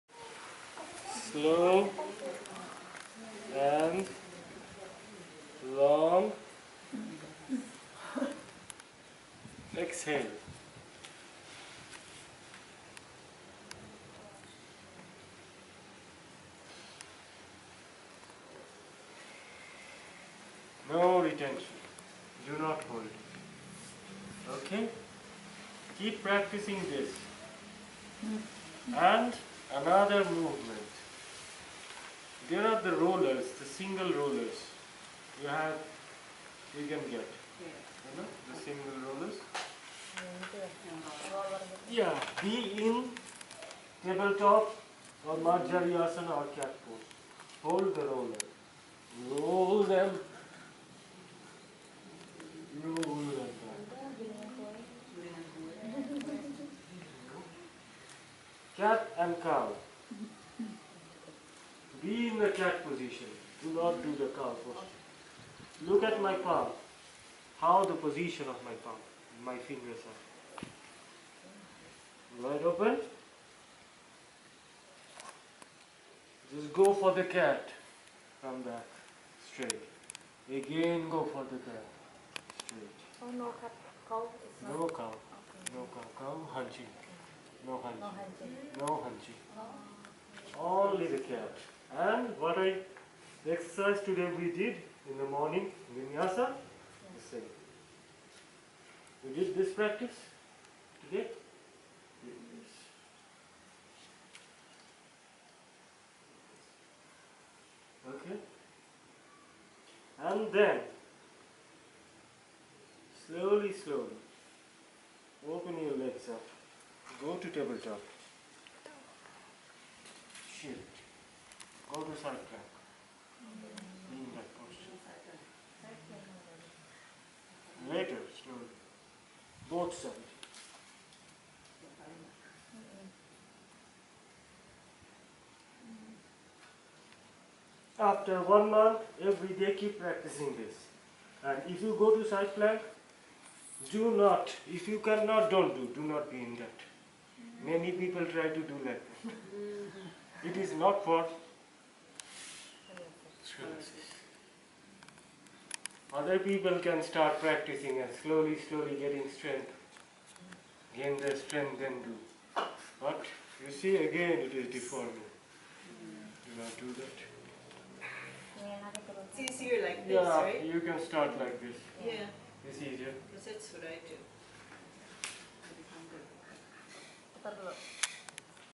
2017 12 03 Yogi Arijit Spinal yoga therapy
how to treat spinal deformation scoliosis and Lordosis.